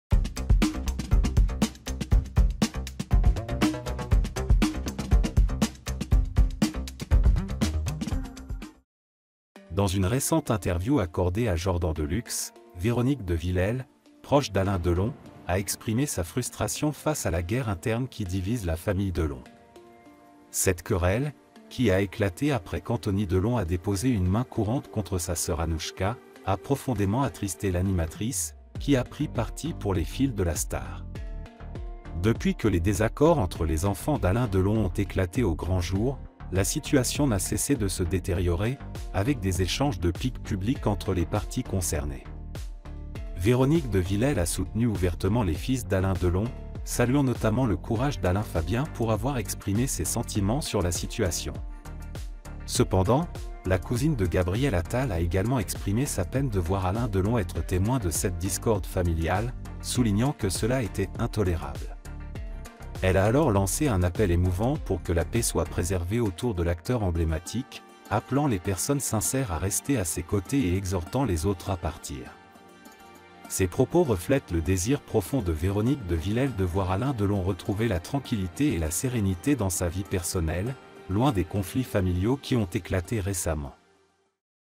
Véronique de Villèle se confie sur son ami Alain Delon : Un cri du cœur touchant !
Dans une récente interview accordée à Jordan de Luxe, Véronique de Villèle, proche d'Alain Delon, a exprimé sa frustration face à la guerre interne qui divise la famille Delon. Cette querelle, qui a éclaté après qu'Anthony Delon a déposé une main courante contre sa sœur Anouchka, a profondément attristé l'animatrice, qui a pris parti pour les fils de la star.